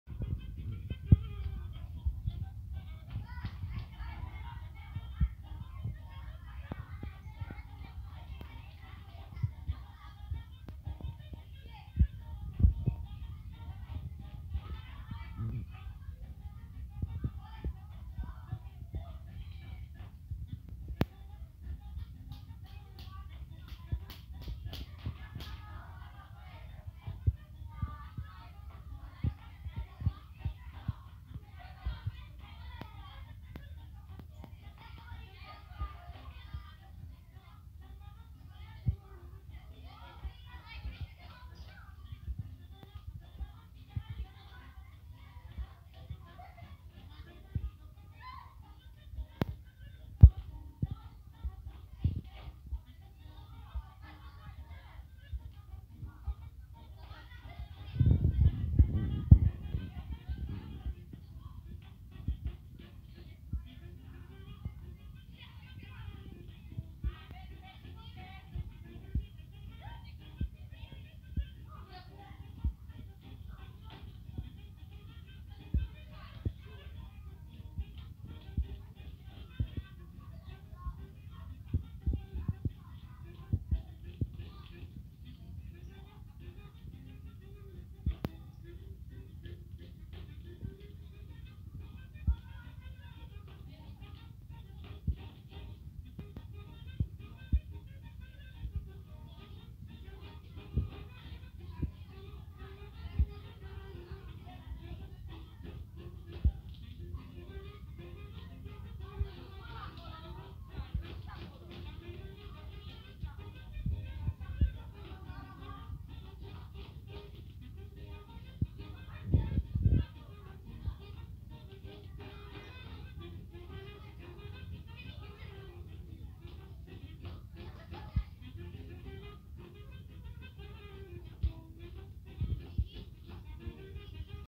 Dansul pinguinului cu doamna învățătoare😢
La revedere cls a-4-a bun venit cls a-5-a la revedere Doamna Lucia 😢😢😢